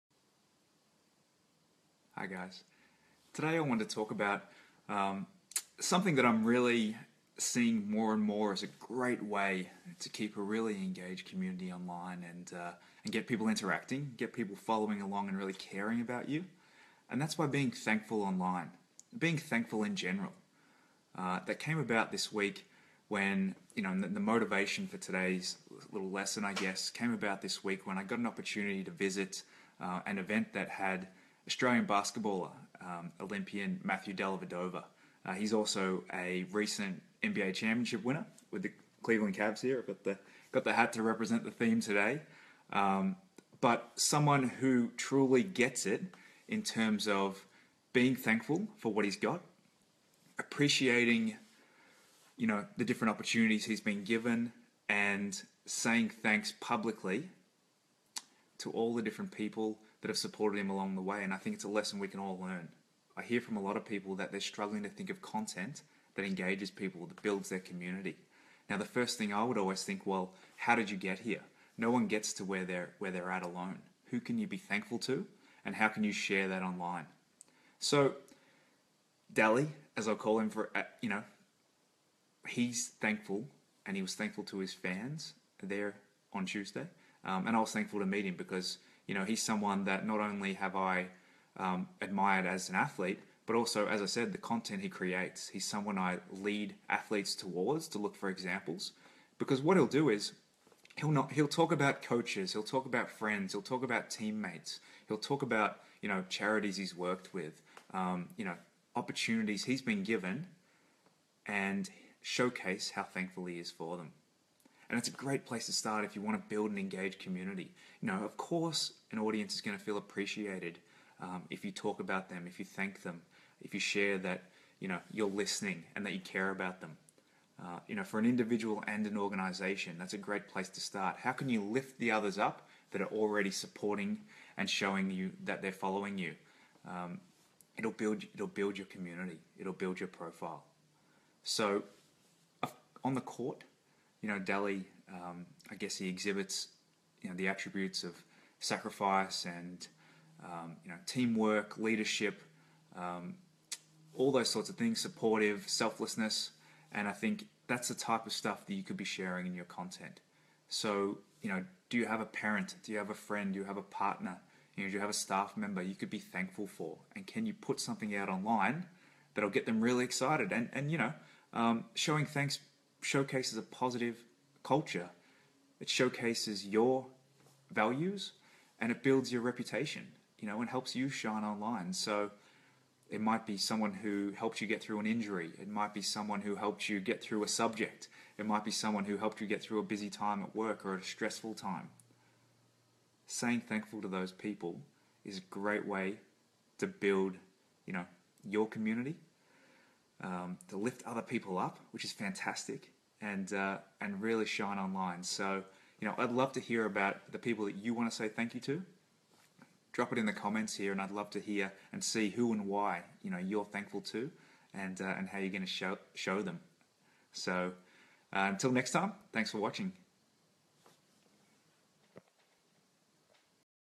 What we can all learn from Matthew 'Delly' Dellavedova about being thankful
A quality character on-court, and on-line.

If you don't follow him on Instagram, check him out. Or visit my collection of athletes 'shining online' (positive examples of social media use) here. He features prominently